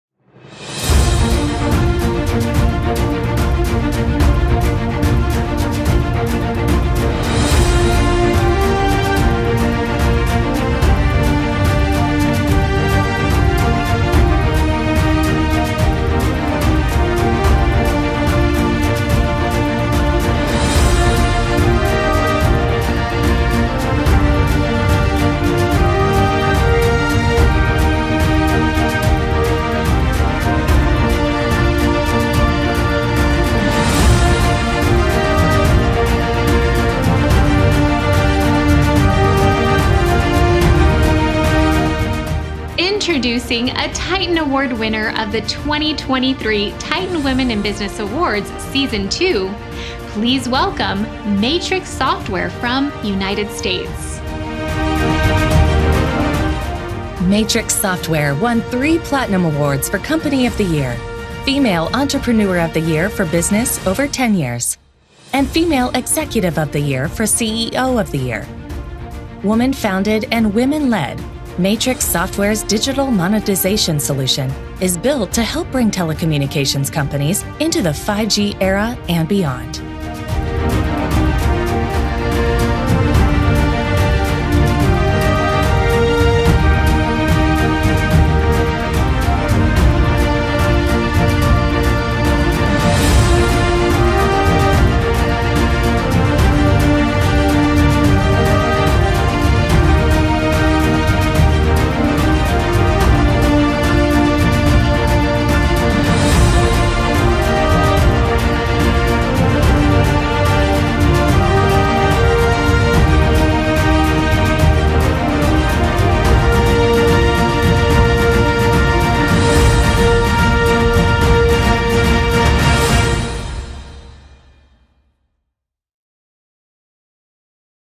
MATRIXX Software - 2023 TITAN Women In Business Awards Season 2
MATRIXX Software won 3 Platinum Awards for Company of the Year, Female Entrepreneur of the Year for Business over 10 Years, and Female Executive of the Year for CEO of the Year. Woman-founded and women-led, MATRIXX Software’s digital monetization solution is built to help bring telecommunications companies into the 5G era and beyond.